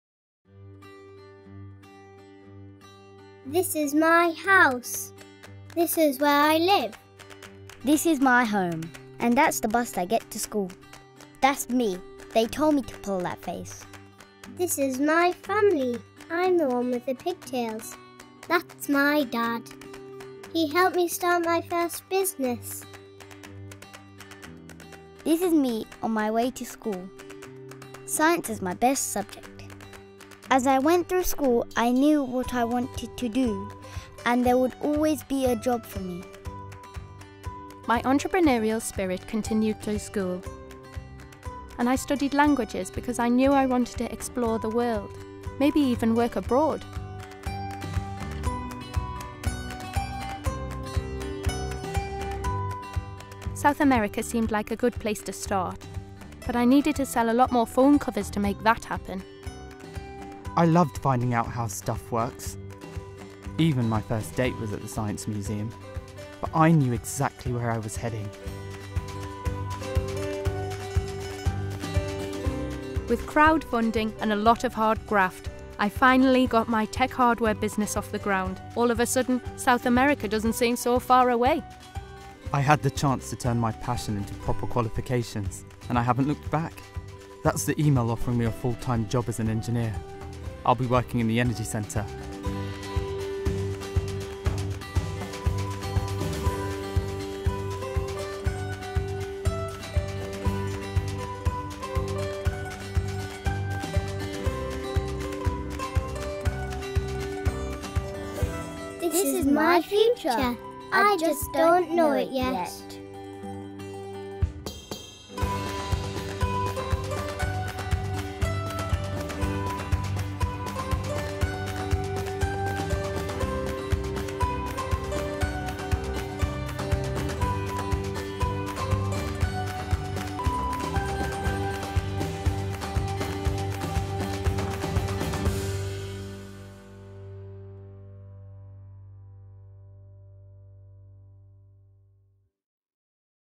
Heathrow Stories of Promise
Heathrow has set out a series of pledges for how expansion would deliver jobs, skills and economic growth for its local communities and the whole of the UK. The five promises, set out in a new report entitled “The Promise of Heathrow,” form part of a larger plan to deliver more than 120,000 new jobs and £100 billion in economic benefits by connecting the UK to global growth.